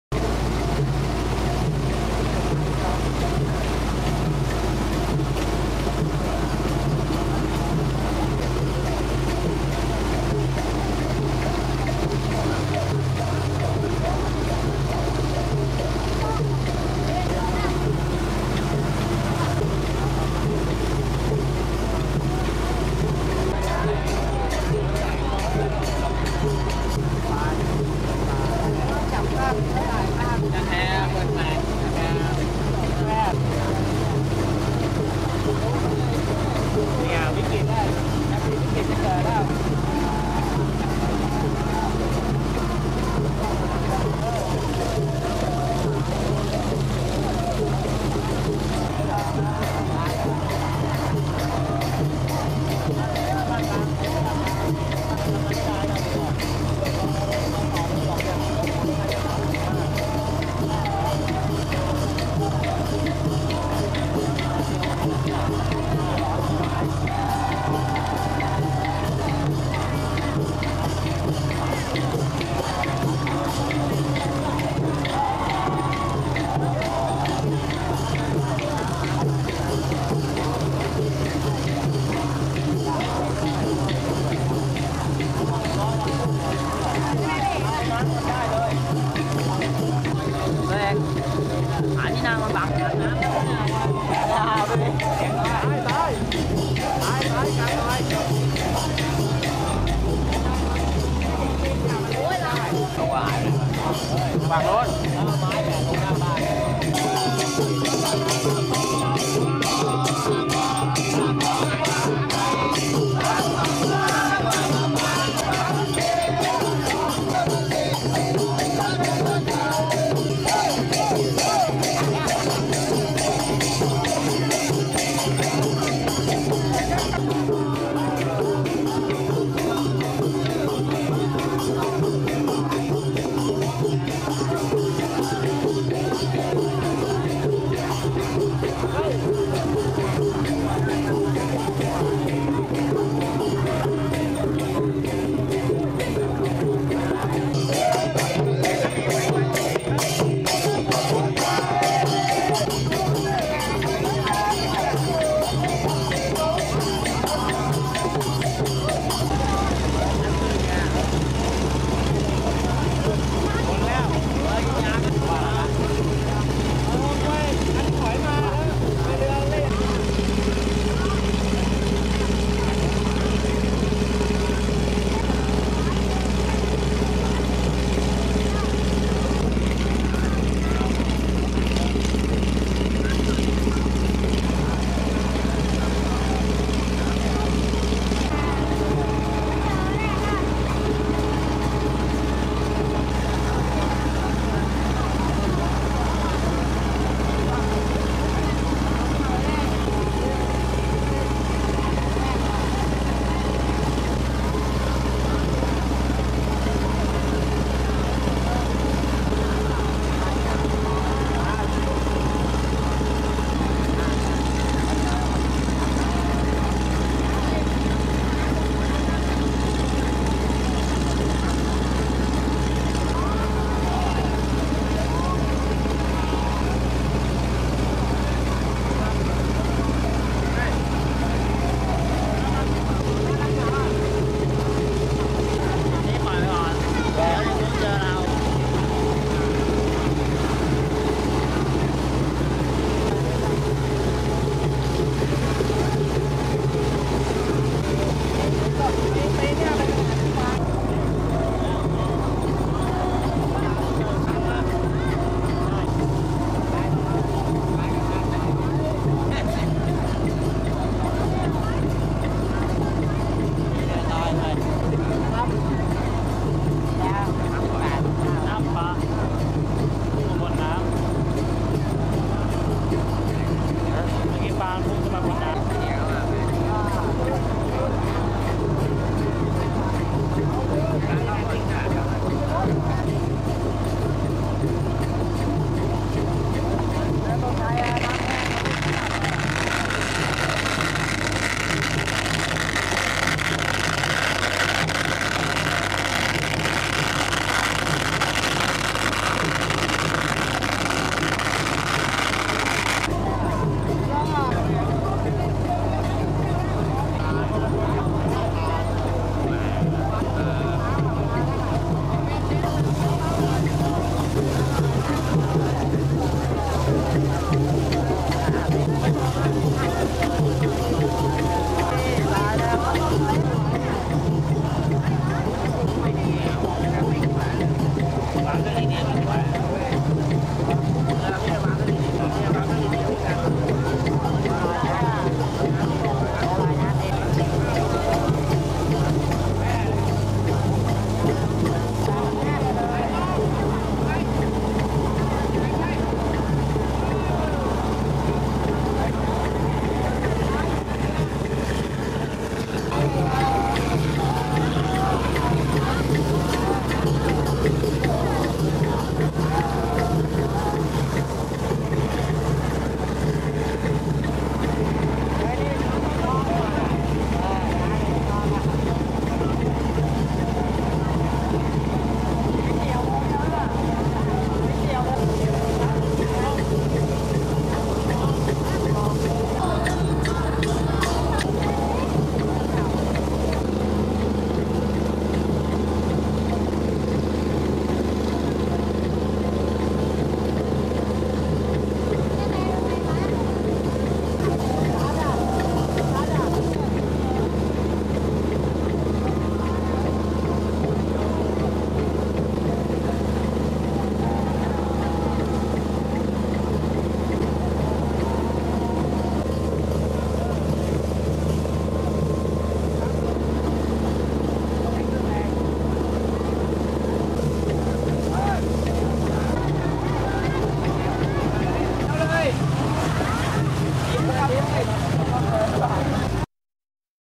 สงกรานต์แห่พระบางปะกง2
งานแห่รูปหล่อหลวงปู่ปานวัดแสมขาว ออกวนทะเล 3 รอบ ในวันสงกรานต์ วันที่ 13 เมษายน 2555  โดยชาวบ้านประมงแสมขาว  สืบทอดมานาน